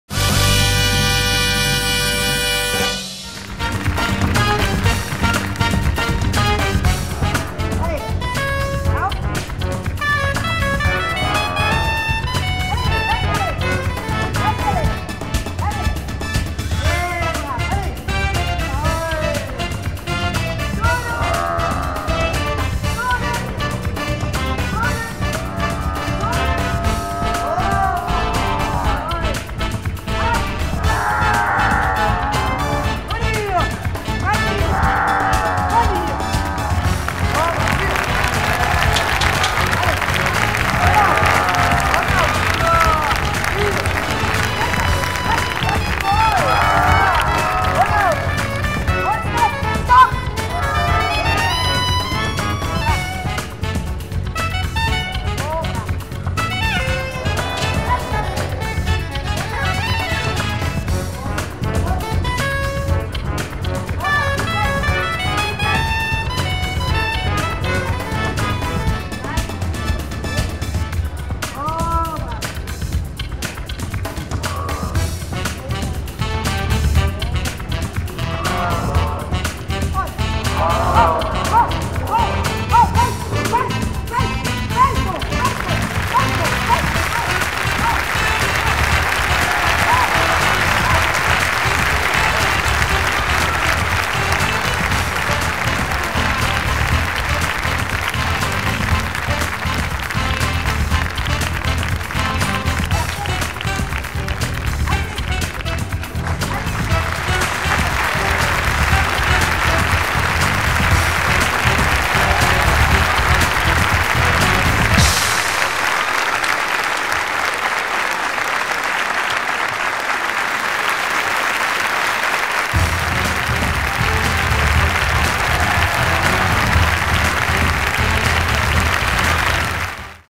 Tigers Carmen Zander 3
Whoever said that only men are the existing ferocious animal tamer surely has not heard of Carmen Zander. She is one of the few female animal tamers around the world. Carmen is known for being a charming and loving tamer. She performed in various TV shows in Germany and participated in the Monte Carlo Circus Festival. Carmen Zander is a winner of her own. At a very young age, she won 15 titles in Rhythmic Gymnastics from 1980 to 1988. In 1991, she was awarded the “Goldenen Elefanten”.